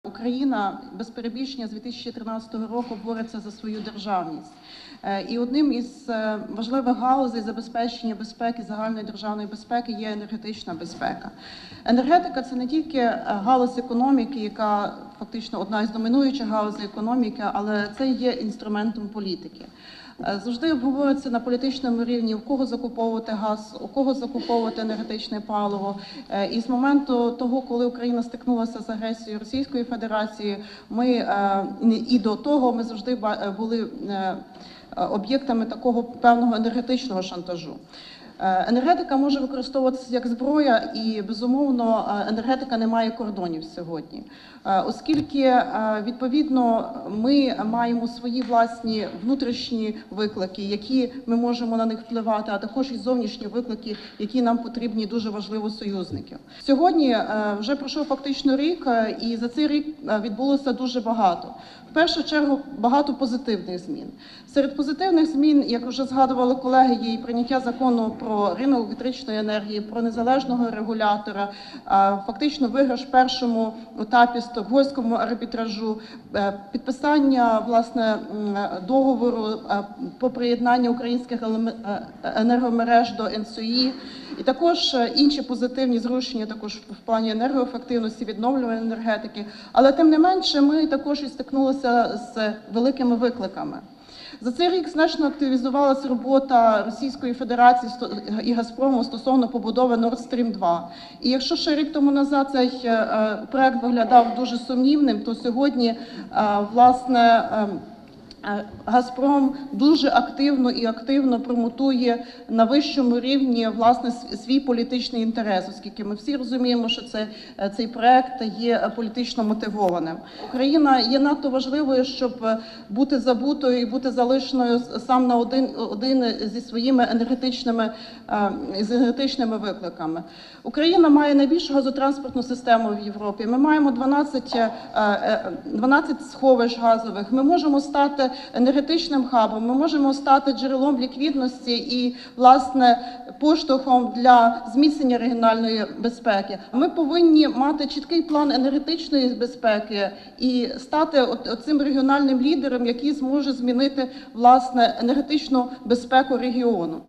Україна має стати регіональним лідером, який змінить енергетичну безпеку, - Н. Кацер-Бучковська
Для того, щоб Україна досягла успіху у питанні енергетичної безпеки, наша держава має бути проактивною у цій сфері, повинна мати чіткий план та стати тим регіональним лідером, який змінить енергетичну безпеку регіону.

Про це заявила представник фракції “Народний фронт”, член Комітету ВР з питань ПЕК, ядерної політики та ядерної безпеки Наталія Кацер-Бучковська під час круглого столу на тему «Ключова роль України в енергетичній безпеці Європи».

Учасники заходу обговорили питання розробки ефективних механізмів та планів дій щодо посилення енергетичної безпеки України та її ролі в забезпеченні енергетичної стабільності Європейського Союзу.

Так, Наталія Кацер-Бучковська зазначила, що “Україна, без перебільшення, з 2013 року бореться за свою державність”. І одним з ключових елементів системи гарантування загальнонаціональної безпеки є, за її словами, саме енергетична безпека.

“Енергетика - це не лише одна з домінуючих галузей економіки, це й інструмент політики. Завжди обговорюється на політичному рівні у кого закуповувати газ, енергетичне паливо. І з того моменту, як Україна стикнулася з агресією Російської Федерації, і до того - ми були об'єктом певного енергетичного шантажу. Енергетика може використовуватися як зброя і, безумовно, енергетика не має кордонів сьогодні”, - заявила народний депутат.

Вона також зауважила, що “сьогодні перед Україною стоять не лише внутрішні виклики, на які ми можемо впливати, але й зовнішні, у яких ми потребуємо надійних союзників”.

Наталія Кацер-Бучковська підкреслила, що питання енергетичної безпеки та енергетичної дипломатії є сьогодні дуже актуальними, тому українські парламентарі та дипломати активно піднімають їх як всередині країни, зокрема на рівні парламенту, так і під час різних важливих міжнародних заходів.

Як повідомила Наталія Кацер-Бучковська, протягом останнього року в енергетичній галузі України відбулося багато позитивних змін. Наприклад, Верховна Рада ухвалила низку важливих енергетичних законопроектів, зокрема про ринок електричної енергії, про незалежного регулятора (НКРЕКП).

Разом з тим народний депутат зауважила, що протягом минулого року Україна також стикнулася і з великими викликами, як, наприклад, активізація побудови Російською Федерацією газогону Nord Stream II в обхід України.

“Якщо ще рік тому цей проект (Nord Stream II) виглядав дуже сумнівним, то сьогодні “Газпром” дуже активно промотує на вищому рівні, власне, свій політичний інтерес. Оскільки ми всі розуміємо, що цей проект є політично вмотивованим”, - наголосила вона.

Як підкреслила Наталія Кацер-Бучковська, не дивлячись на те, що українська влада вже зробила чимало для розвитку енергетичного сектору, тим не менш “Україна ще не перебуває на тій стадії, коли реформи є незворотними”. Тому, на її думку, необхідно завершити розпочаті реформи в енергетичному секторі, зокрема має відбутися розподіл НАК “Нафтогаз”, мають бути ухвалені зміни до законодавства для збільшення видобутку газу в Україні тощо.

“Україна є надто важливою, щоб бути забутою та залишеною один на один зі своїми енергетичними викликами. Україна має найбільшу газотранспортну систему в Європі, ми маємо 12 газових сховищ, ми можемо стати енергетичним хабом, ми можемо стати джерелом ліквідності, поштовхом для зміцнення регіональної безпеки”, - заявила представник фракції “Народний фронт”.

Більше того, на думку парламентаря, для того, щоб Україна досягла успіху у питанні енергетичної безпеки, наша держава має бути проактивною.

“Ми повинні мати чіткий план енергетичної безпеки і стати тим регіональним лідером, який може змінити, власне, енергетичну безпеку регіону”, - підсумувала Наталія Кацер-Бучковська.

У зв'язку з цим, як вона повідомила, парламентарі спільно з експертами Інституту суспільно-економічних досліджень наразі розробляють «План заходів зі зміцнення енергетичної безпеки України до 2020 року», а також у Верховній Раді готується законопроект про засади енергетичної безпеки.

Приєднуйся!